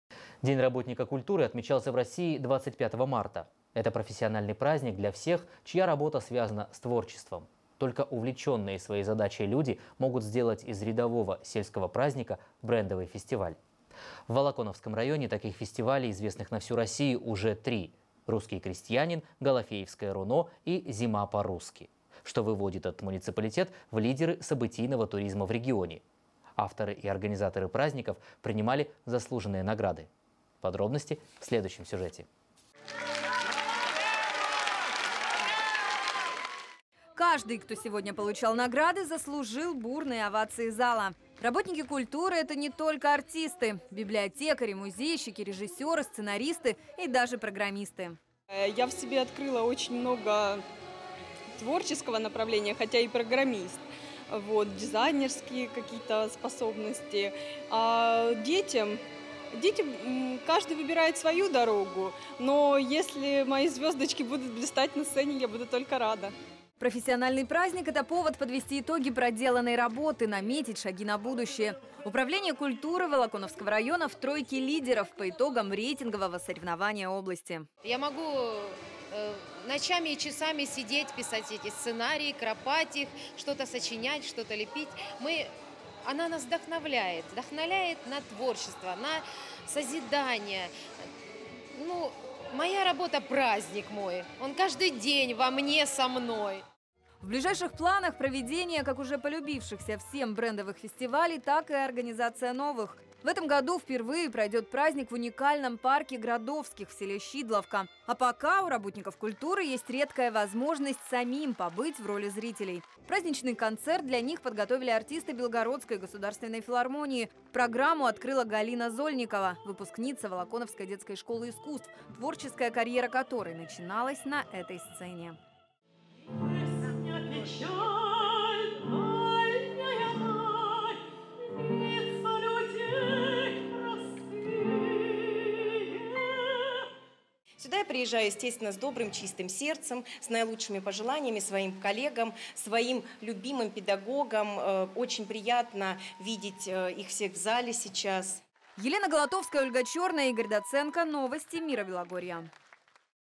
Лучшие работники культуры в Волоконовке
25 марта в России отмечался День работника культуры. Это профессиональный праздник для всех, чья работа связана с творчеством. Только увлеченные своей задачей люди могут сделать из рядового сельского праздника брендовый фестиваль. В Волоконовском районе таких фестивалей, известных на всю Россию, уже три: «Русский крестьянин», «Голофеевское руно», «Зима по-русски». Авторы и организаторы  этих праздников принимали заслуженные награды. Подробности – в следующем сюжете.